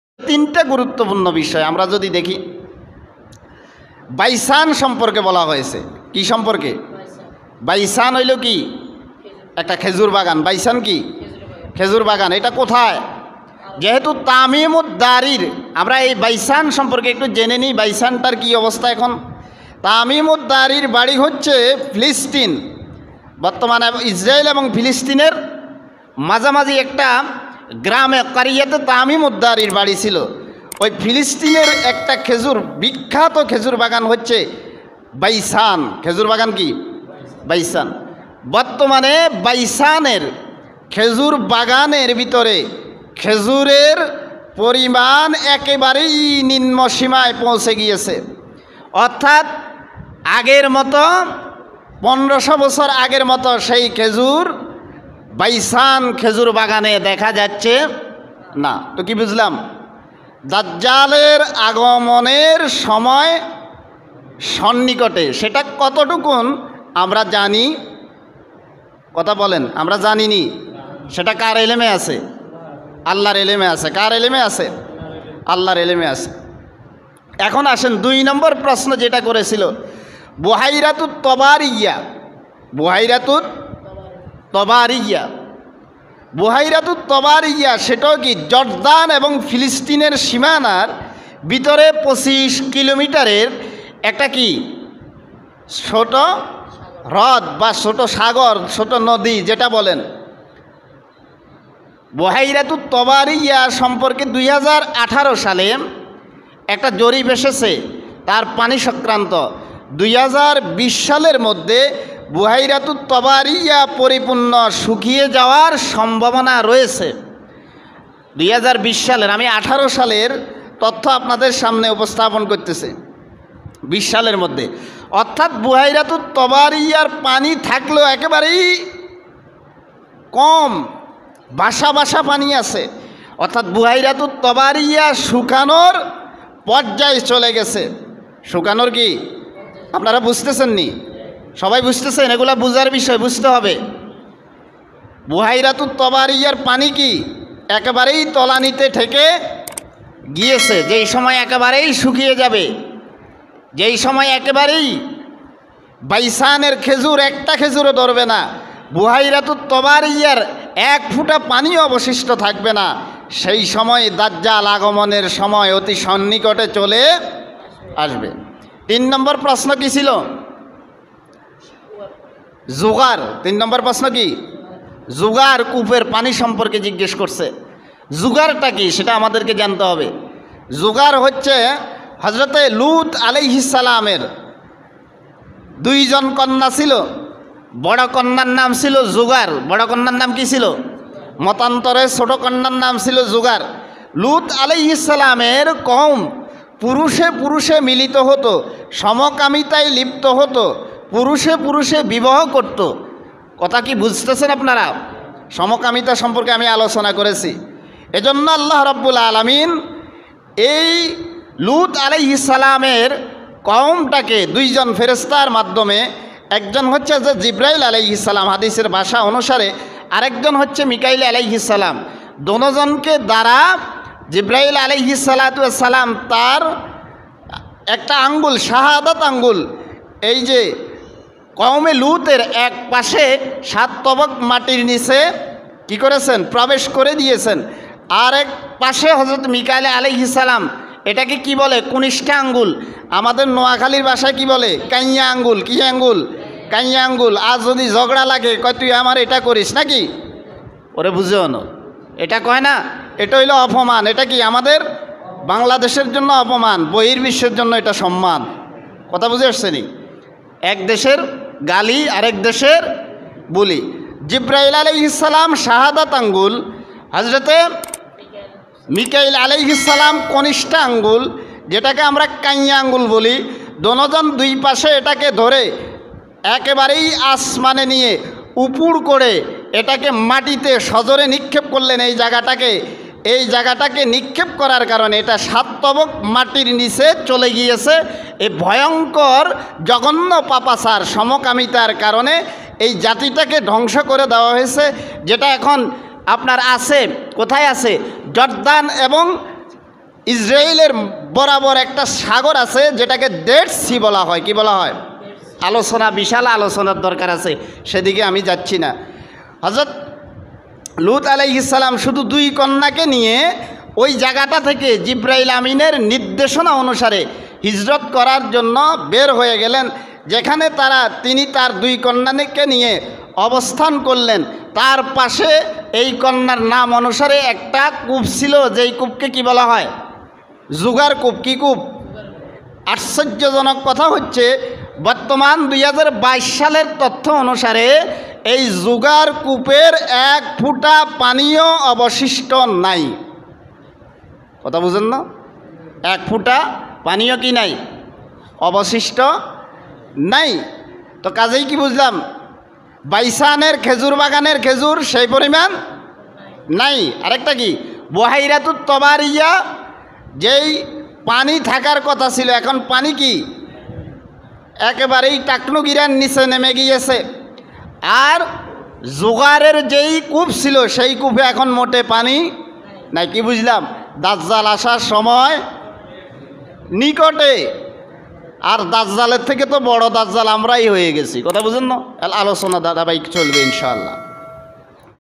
বাইসান,বুহাইরাতুত তবারি ও জুগার কূপের কী অবস্থা?
দাজ্জালের জানতে চাওয়া বাইসান
বুহাইরাতুত তবারি ও জুগার কূপের
বর্তমান কী অবস্থা।

বিষয়ভিত্তিক আলোচনা।

মুফতী মুহাম্মাদ ইব্রাহীম।প্রভাষক(আরবি)ওসমানিয়া আলিম মাদ্রাসা।
সোনাগাজী-ফেনী।
খতিবঃ গোপাই জামে মসজিদ(সদর-নোয়াখালীত)।

দলিলঃমুসলিম(৭২৭৬)নম্বর হাদিস।

কোরআন ও হাদিসের অসংখ্য দলিল প্রমান উপস্থাপন করা হয়েছে।

আসুন সবাই মিলে কোরআন সুন্নাহর আলোকে জীবন
গড়ি।

ইনশাআল্লাহ।